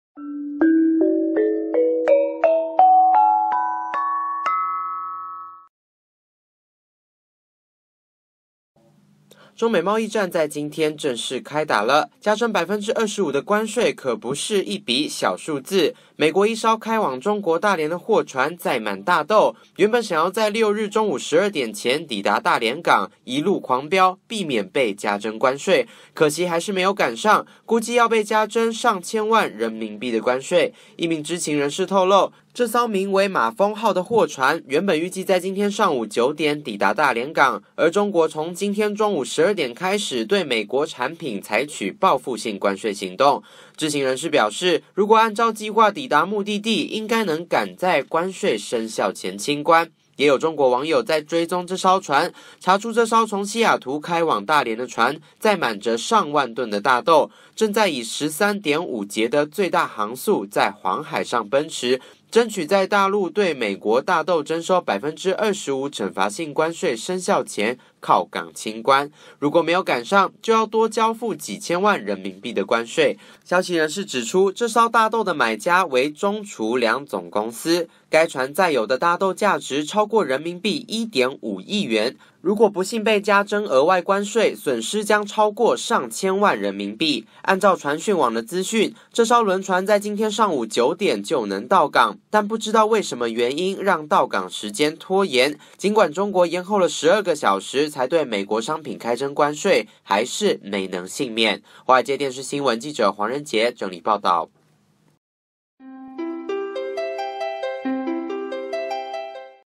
不想多付关税，美国大豆船高速飙向中国（《华尔街电视新闻》2018年7月6日）
中美贸易战在今天正式开打了，加征25%的关税可不是一笔小数字，美国一艘开往中国大连的货船，载满大豆，原本想要在6日中午12点前抵达大连港，一路狂飙，避免被加征关税，可惜还是没有赶上，估计要被加征上千万人民币的关税。